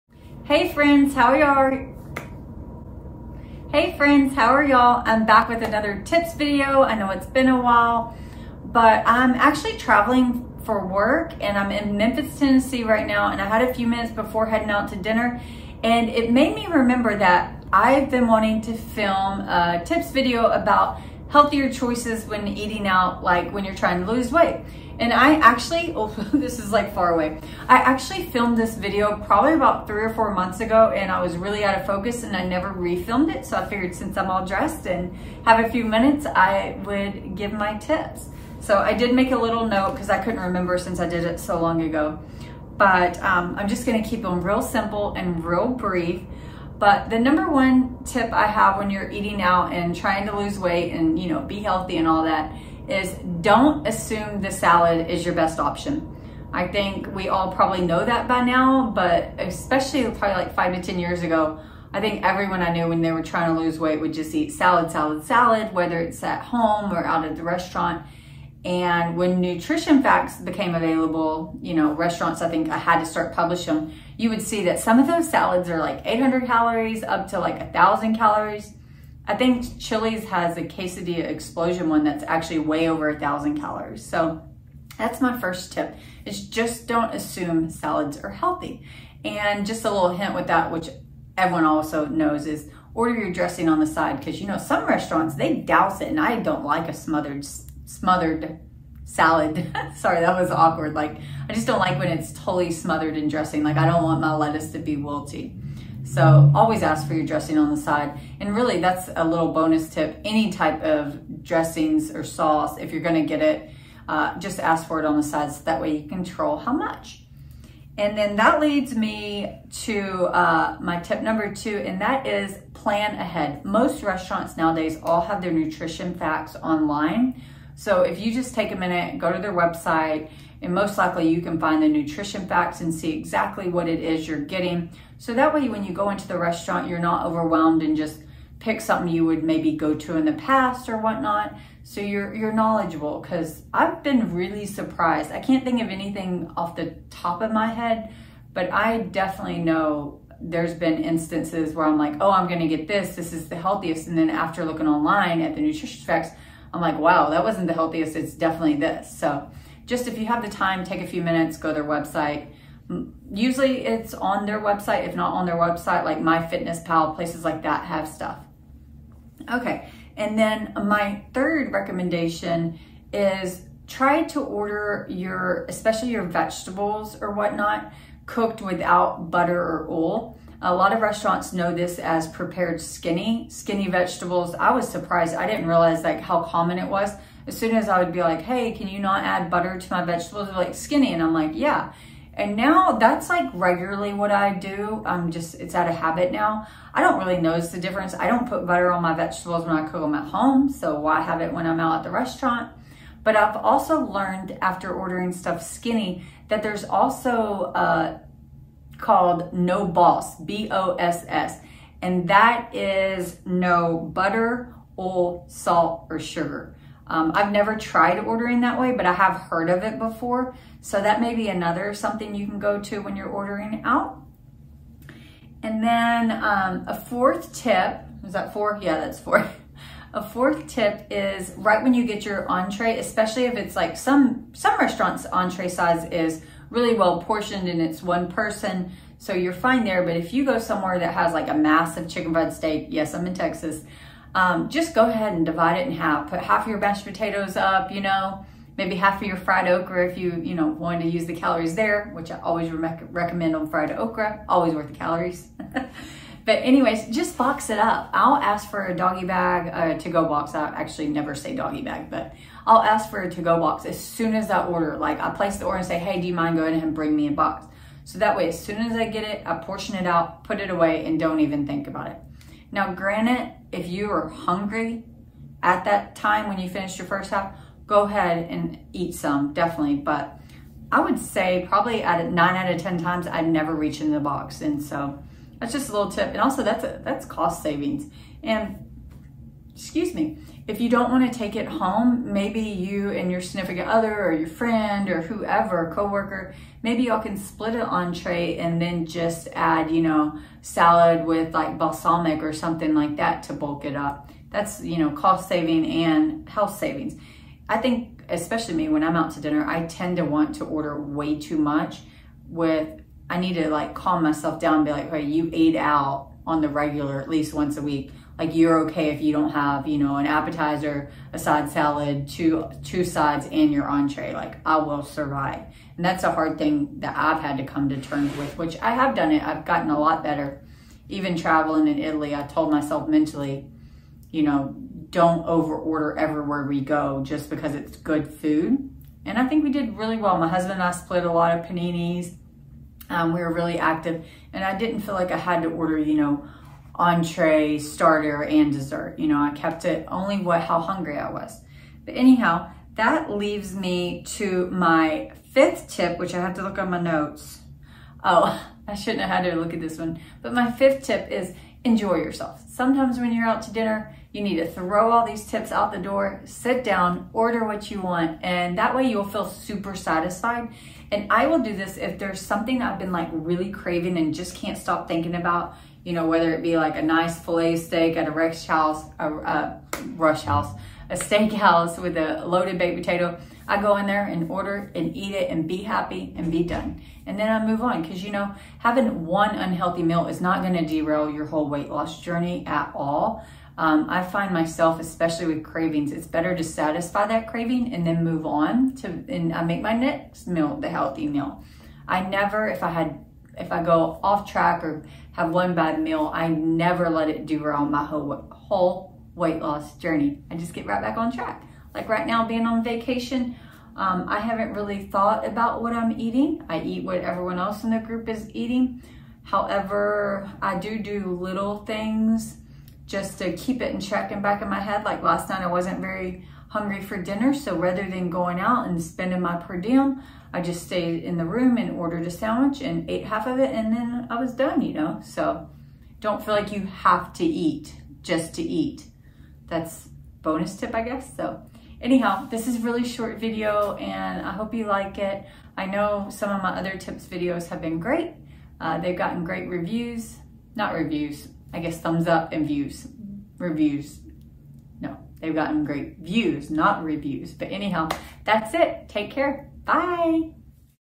5 Tips For Eating Out While On A Diet
How to eat out and still lose weight. Tips for successful weight loss.

In today's video I'm going to share my top 5 tips for eating out while trying to lose weight. I really wish I would have included that trying not to eat out as much as possible is  advantage when trying to lose weight .

As  far as my weight loss journey I've had success on WW. I'm 5'7", 41 years old  and currently down 42 pounds.

I hope you find these tips helpful and encouraging. Cheers!